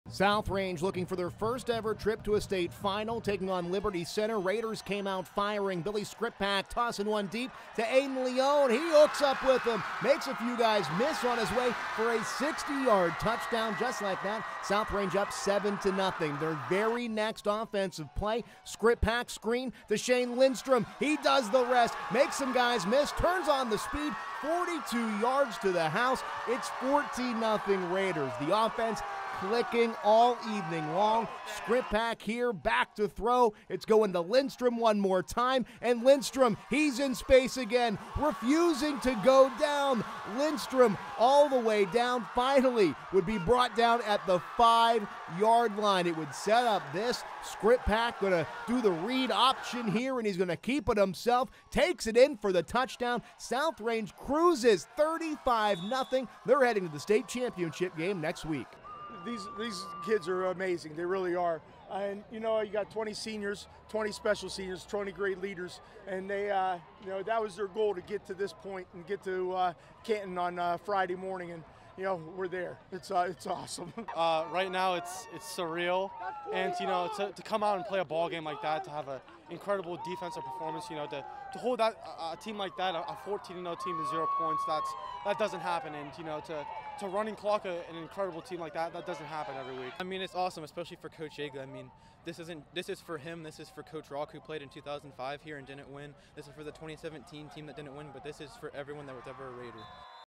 South Range stays perfect, bound for state title game
The Raiders advance to their first football state championship game, remaining perfect on the season with a record of 15-0